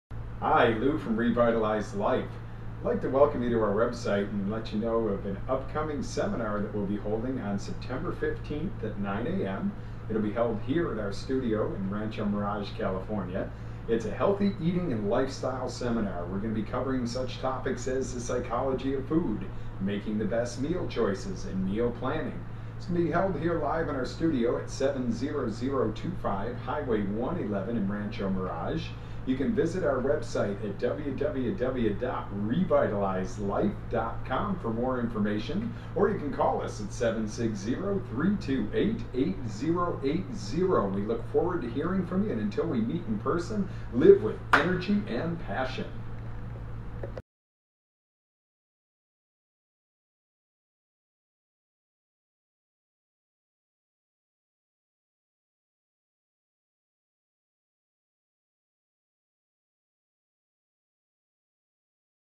Healthy Eating and Lifestyle Seminar Palm springs Rancho Mirage
You are invited to join us on Saturday September 15th at 9AM for a live seminar Healthy Eating and Lifestyle.